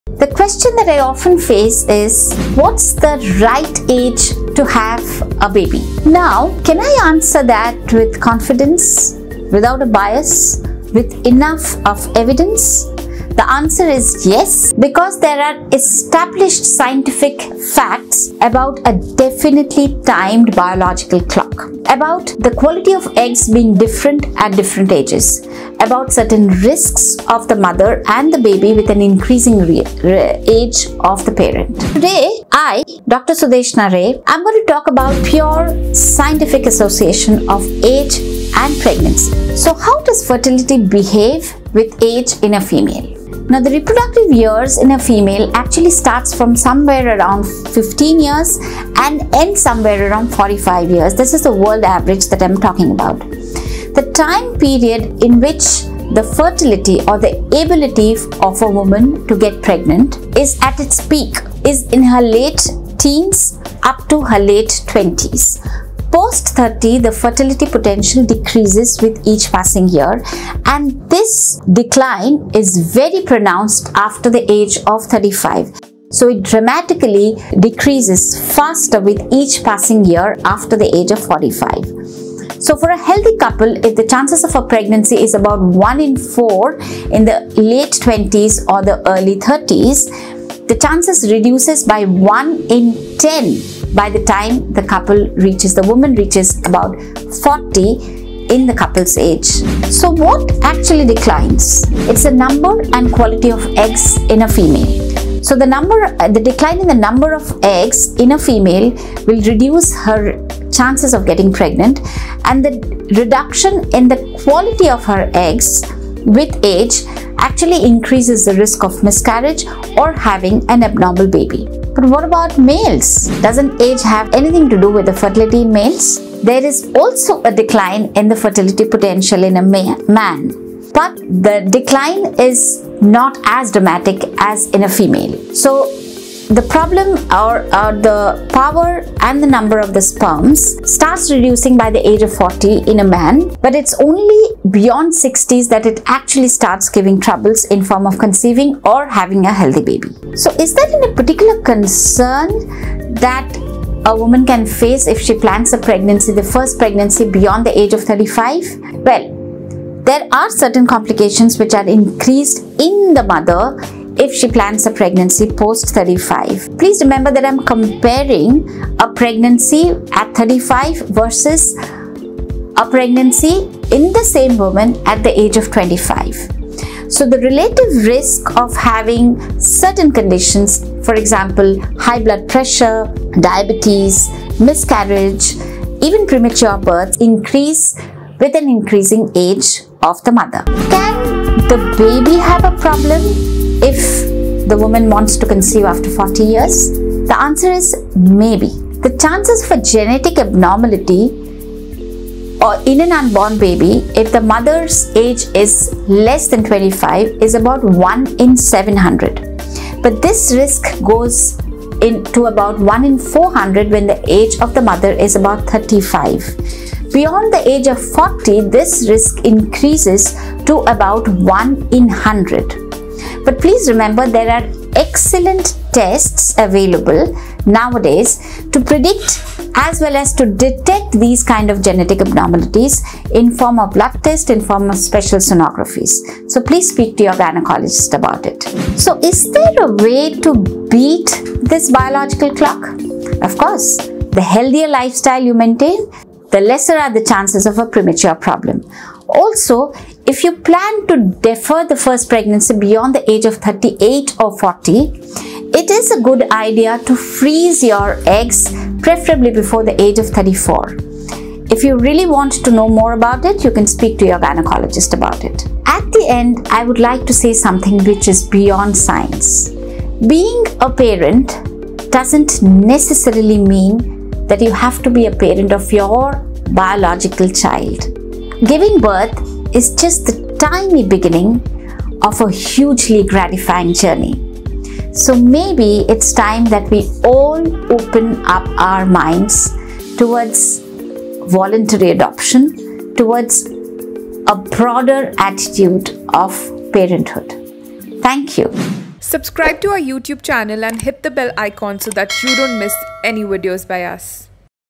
What's the right age to get pregnant? | Explains Dr. Sudeshna Ray
Dr. Sudeshna Ray, Senior Consultant Obs & Gyn answers explains what is the right age for a female to get pregnant?

Discussed in this:
→How does a woman's biological clock work?
→Most fertile years for a woman and a man
→How is age related to pregnancy?
→How does fertility behave with age in a female?
→What are the problems that arise when you plan to get pregnant post 35?
→Can you beat your biological clock?

FIND OUT!